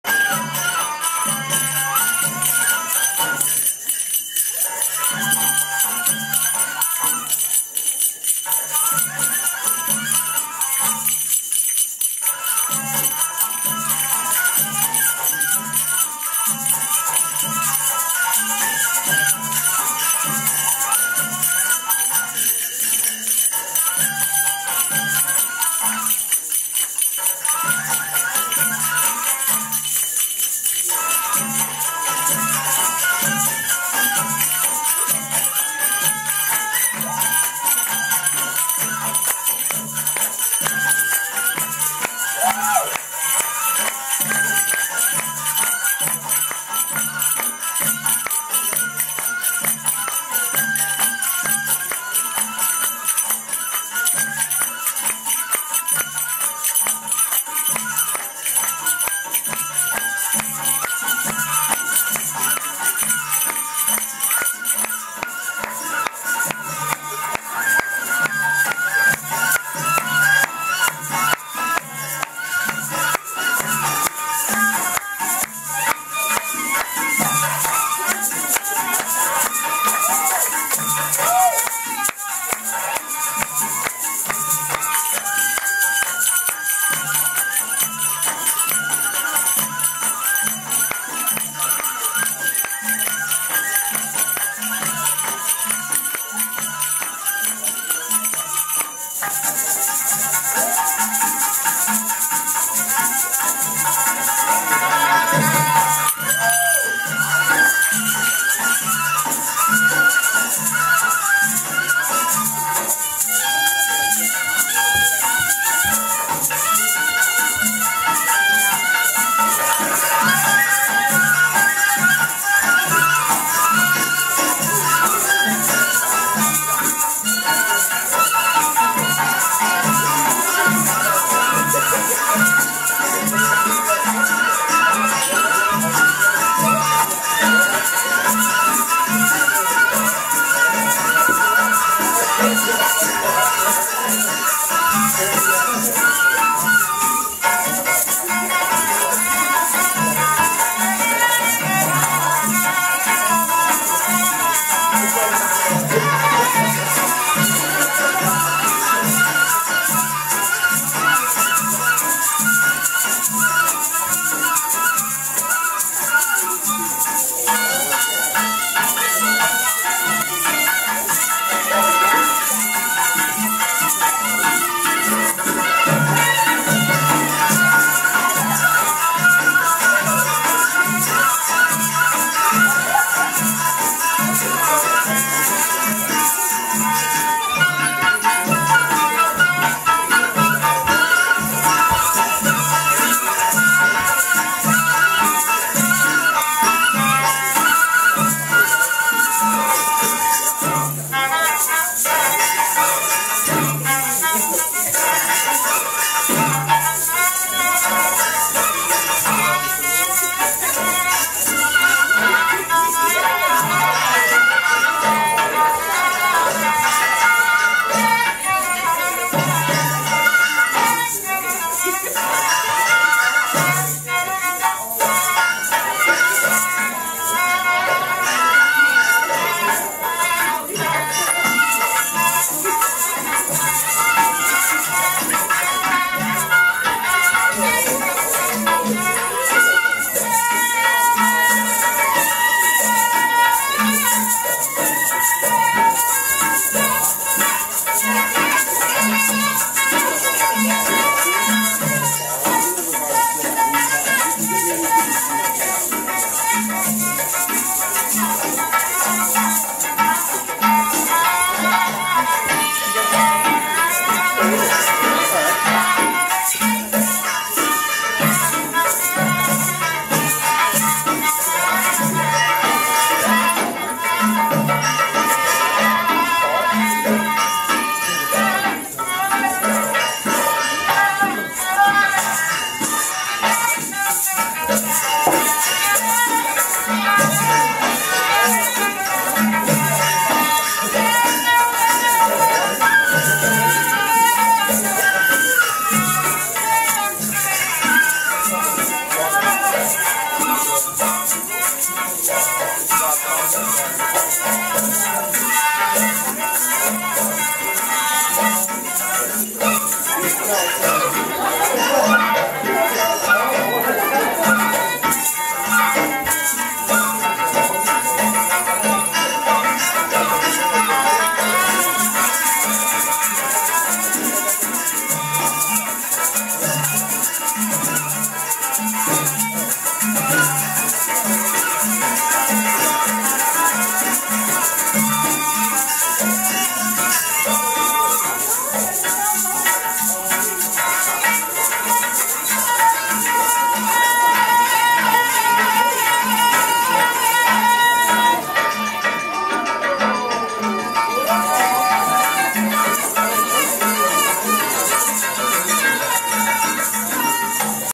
Ghawazee Ghawazi dance folk belly dance Egypt - RALUCA GHIBAN
improvised dance ( the camera went out of battery...lol)